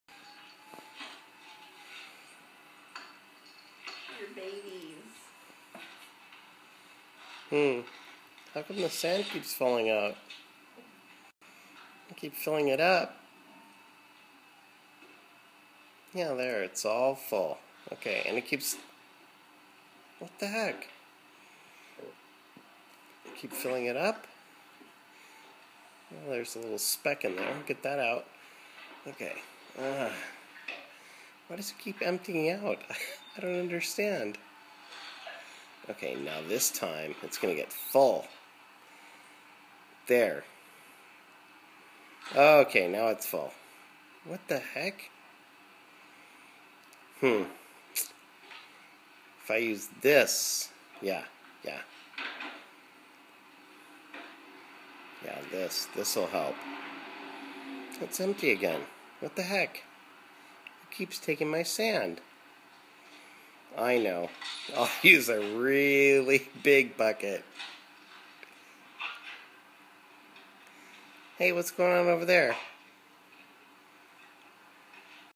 Jennifer tries to fill her bucket with sand...tries.
Jennifer tries to fill her bucket with sand and keeps dumping it out herself. This is from home VHS video tapes.